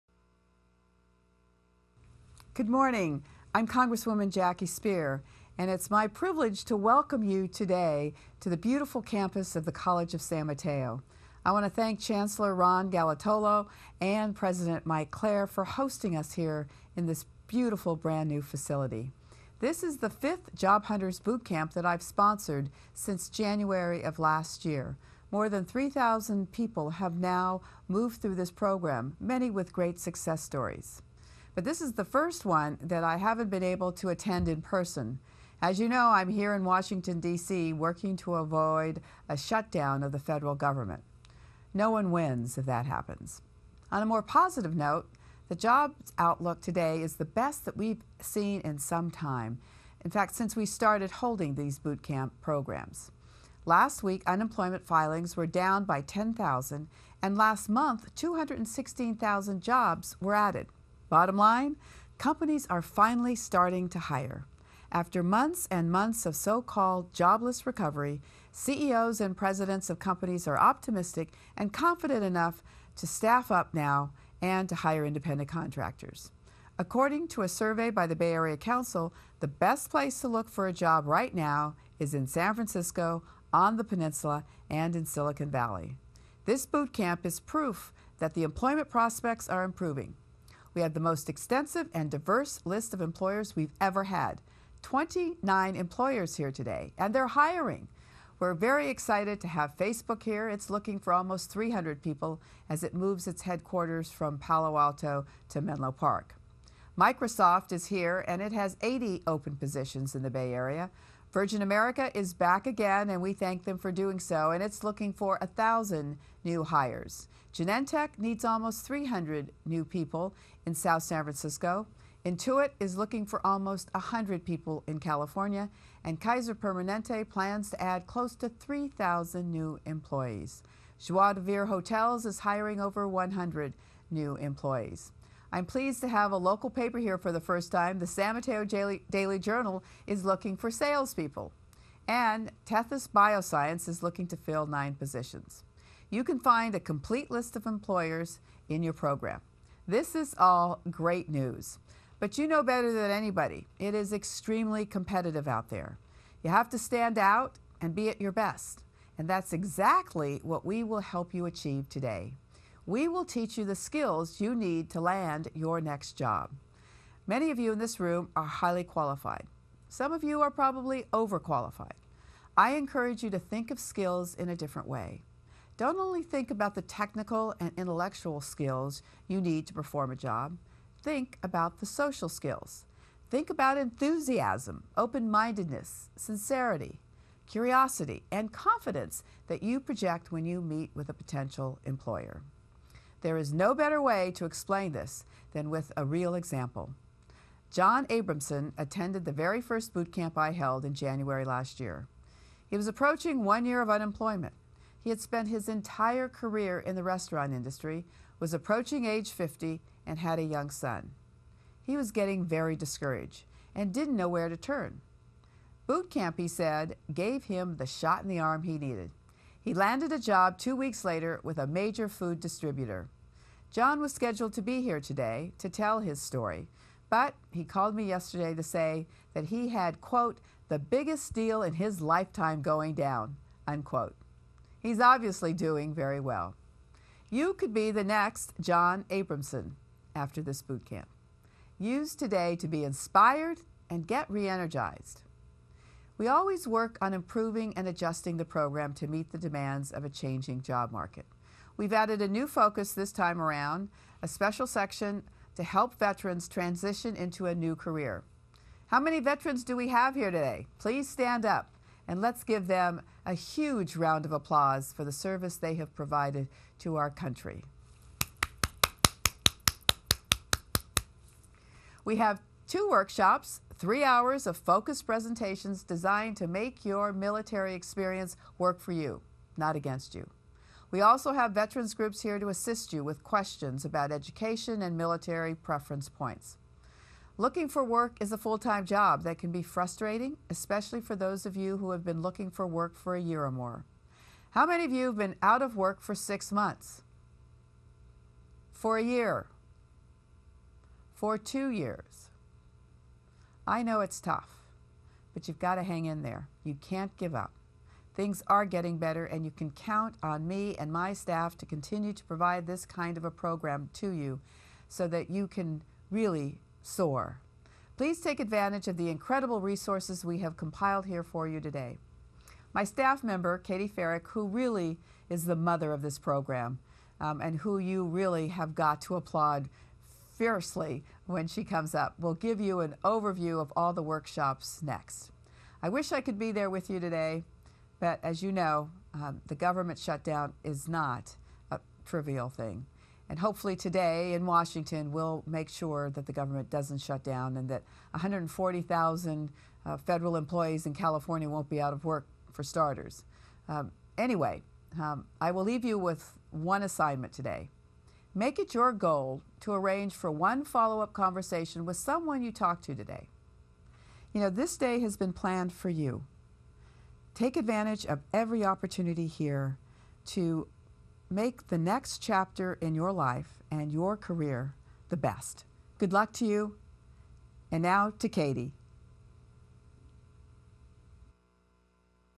JHBC 4.8.11 Jackie's opening remarks .mpg
Job Hunters Boot Camp, April 8, 2011, opening remarks from Congresswoman Speier.